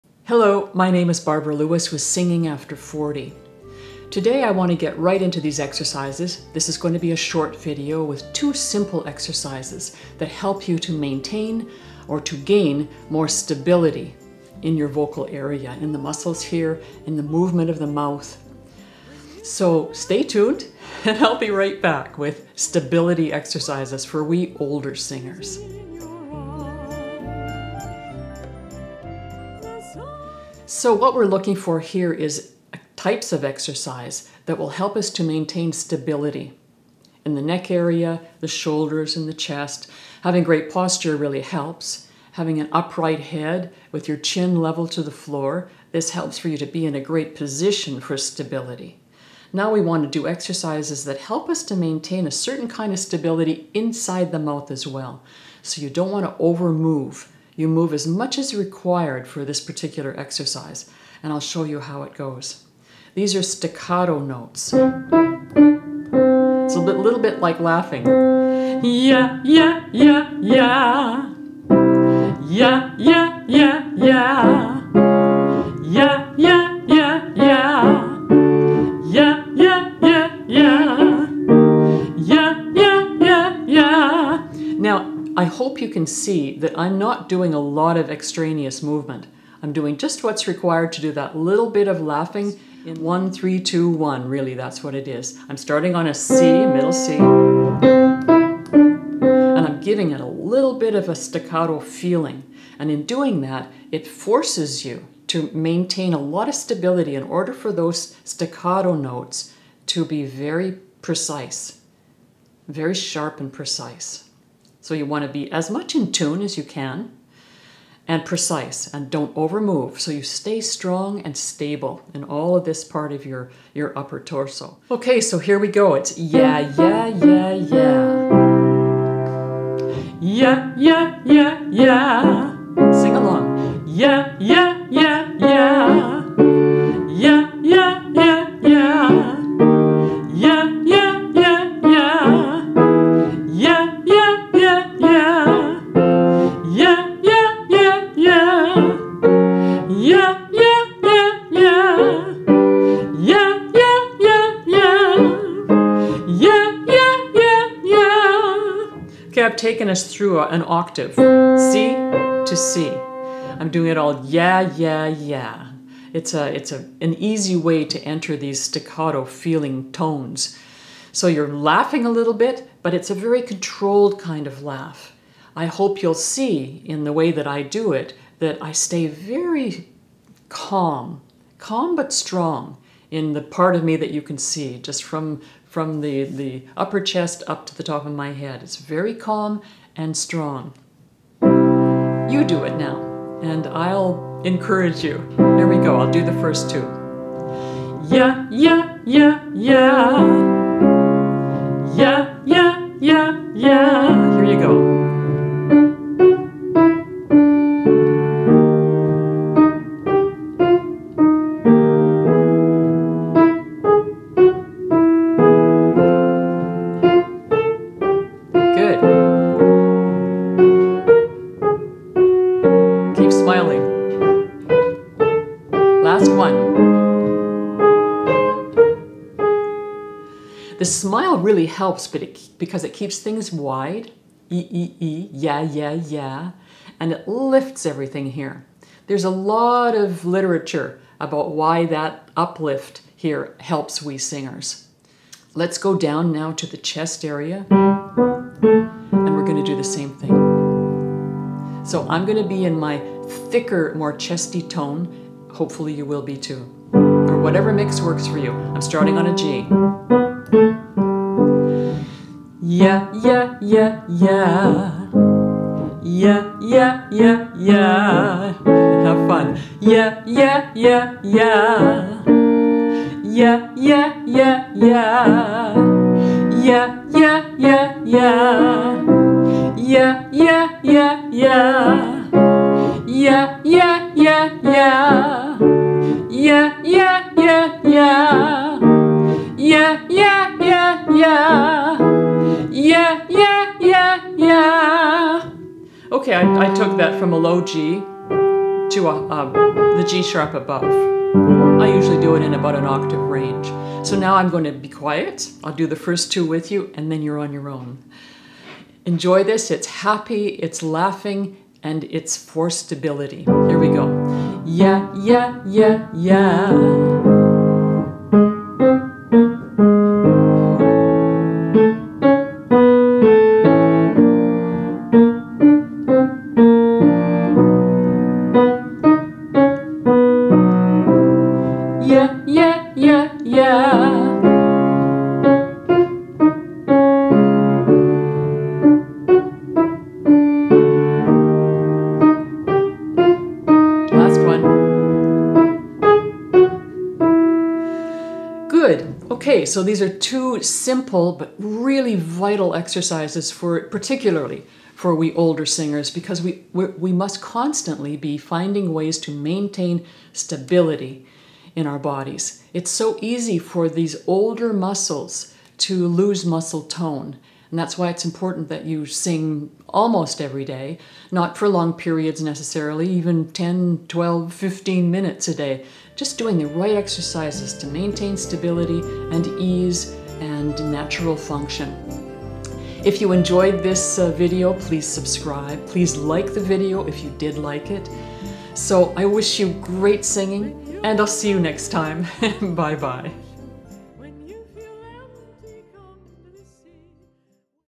I want a younger voice! Tips for a younger voice. Singing After 40 with Barbara Lewis
Barbara Lewis teaches singers over 40 how to sing well!

Or you want to sing a special song better!

Read my column about confidence building here:
Confidence Boosters - Uncommon Ones - Especially for Singers